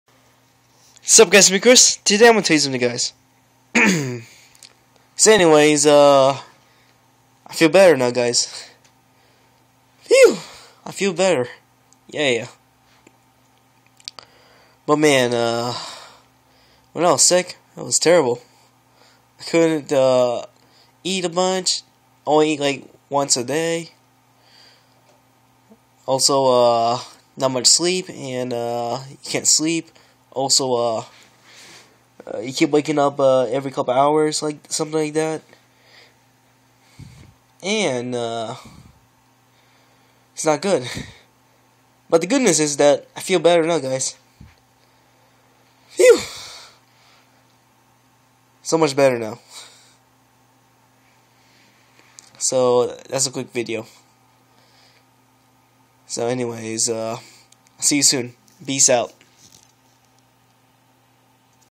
Vlog: Feel better now
Man thank goodness I'm in normal health again. I also want to thank my true friends that are with me when I came back for school. They trust me and sit nearby me, I respect that. Thank you.